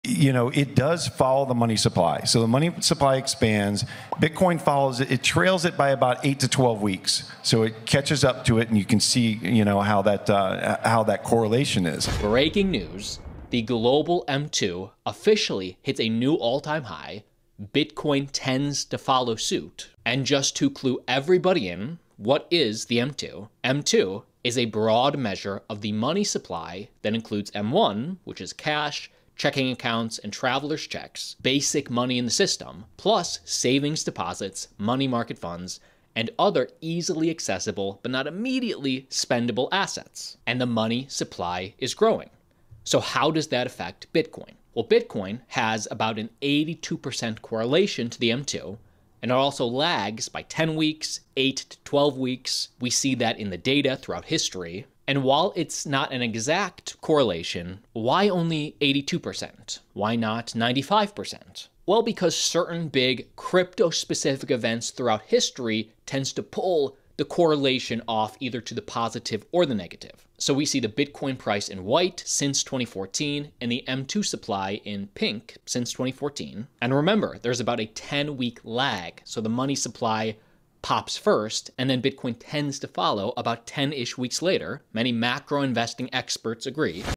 🚀 Bitcoin's Surprising Link to M2 Money Supply! 📈
Join us as we dive into the fascinating relationship between Bitcoin and the M2 money supply. Discover how this correlation could indicate a thrilling rise for cryptocurrency, driven by macroeconomic trends!